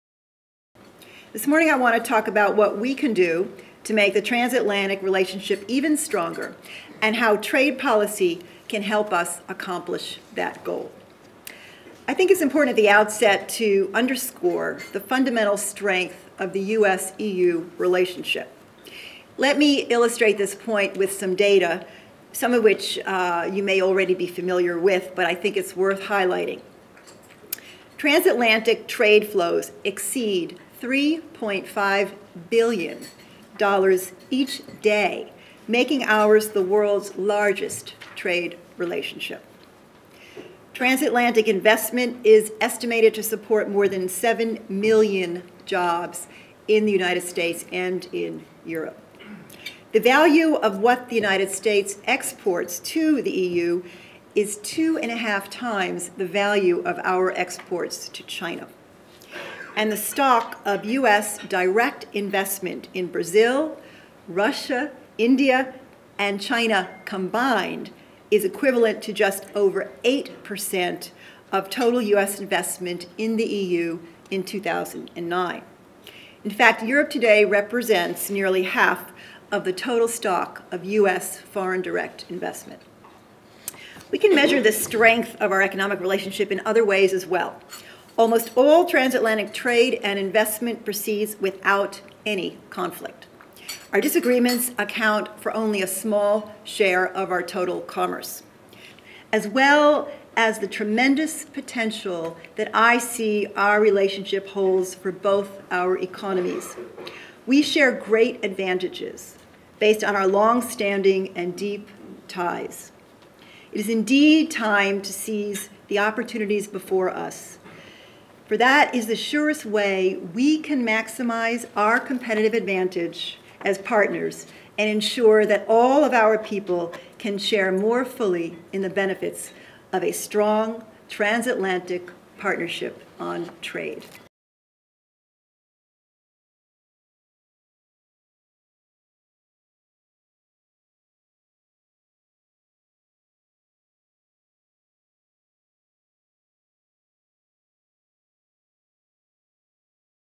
Deputy USTR Sapiro on the fundamental strength of the U.S.-EU trade relationship
Video filming & editing: USEU/Office of Public Affairs, Brussels, Belgium.

The Deputy U.S. Trade Representative Miriam E. Sapiro participated to a briefing on the transatlantic trade relationship organized by the European Policy Centre and the U.S. Mission to the EU on February 10, 2011.

In this video excerpt Deputy U.S. Trade Representative Miriam E. Sapiro underscores with data and metrics the fundamental strength of the U.S.-EU trade relationship. "Transatlantic trade flows exceed 3.5 billion dollars each day making ours the world's largest trade relationship."